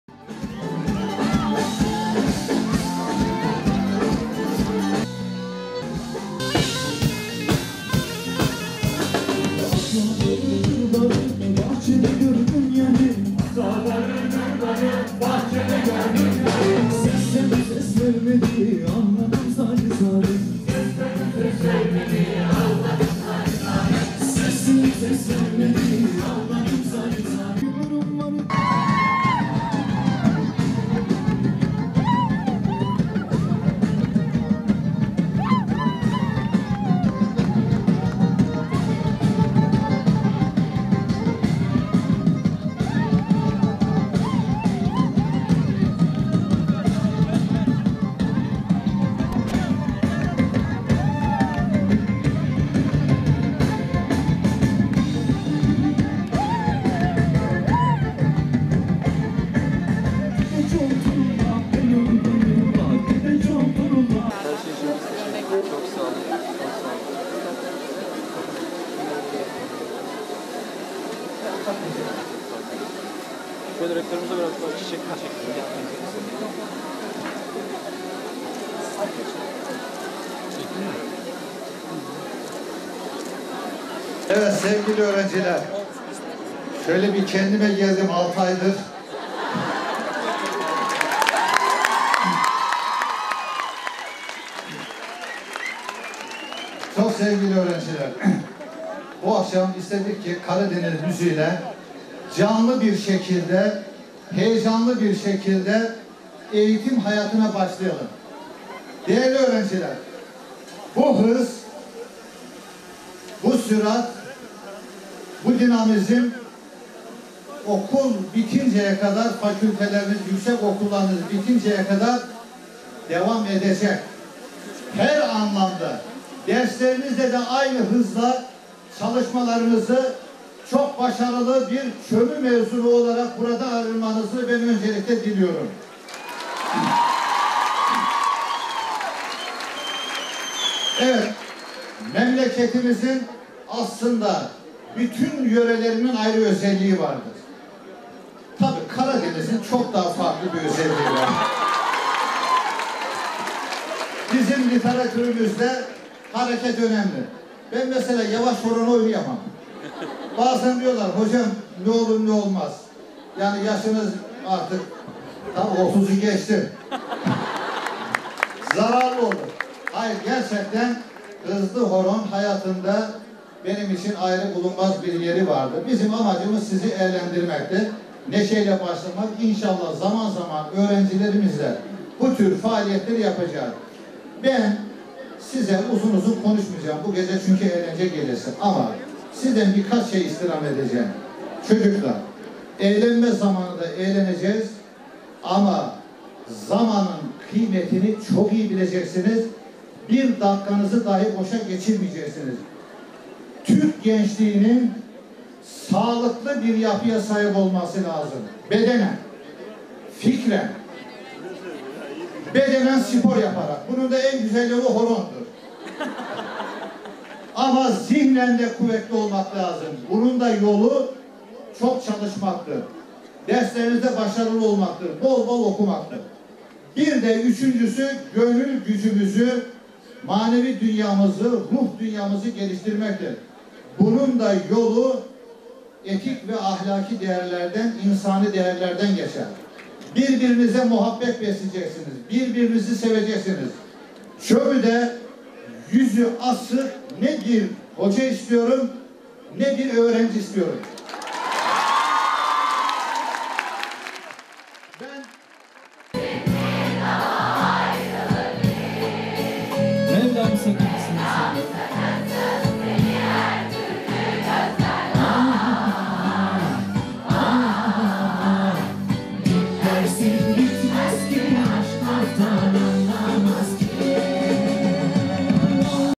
KOLİVA GRUBU ÇOMÜ’LÜ ÖĞRENCİLER İLE BULUŞTU
Karadenizli müzik grubu Koliva, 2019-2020 Eğitim-Öğretim Yılı açılışı dolaysıyla ÇOMÜ’lü öğrenciler ile buluştu.